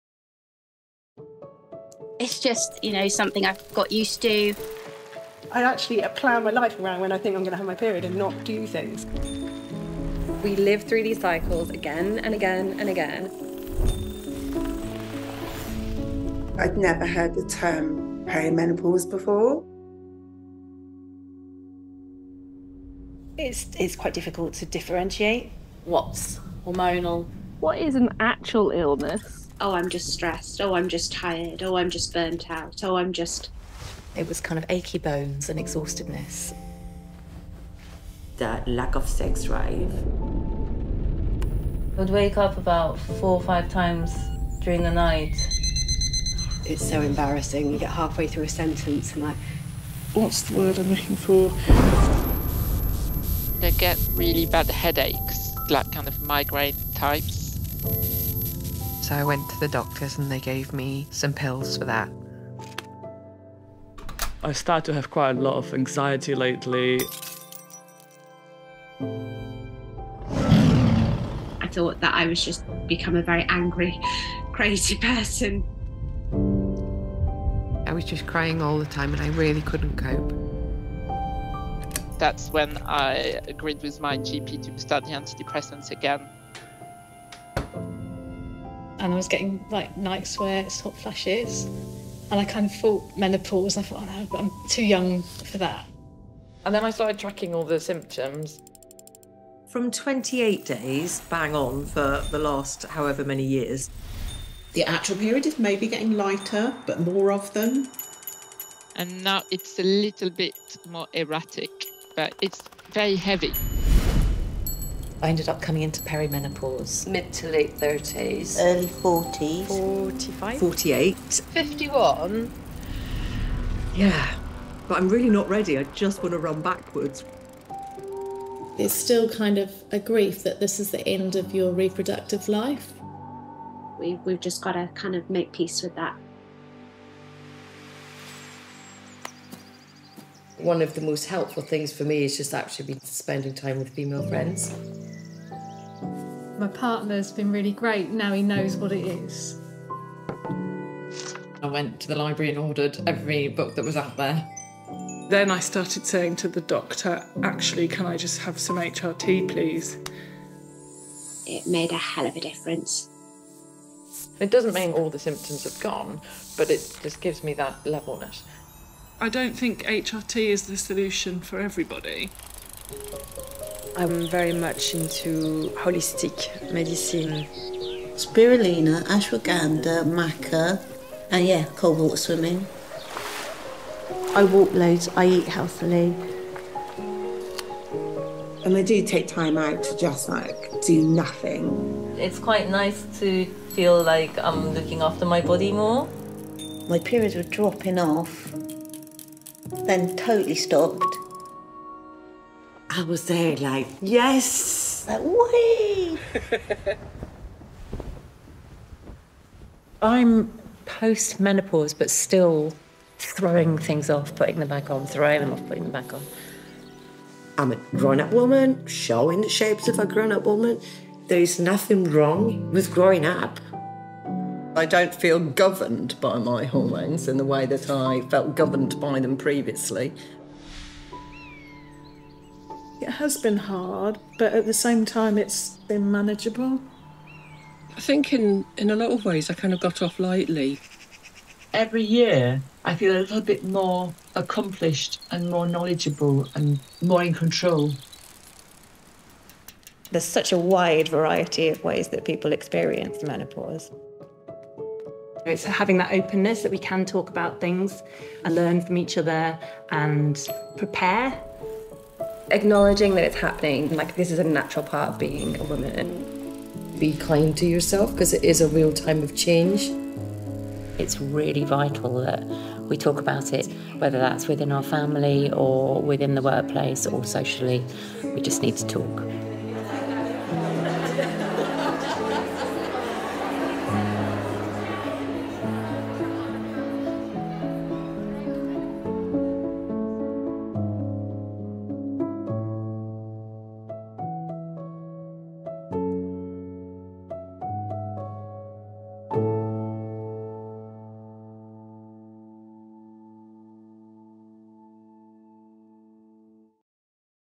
Cycle of Change; Understanding Menopause
Cycle of Change: Understanding Menopause is a short film to empower women and share real-life experiences from all stages of the menopause transition. By dispelling the prevailing myths and misunderstandings, this film seeks to educate women living through the experience, as well as educators, employers and healthcare providers. We hope it helps women across the world.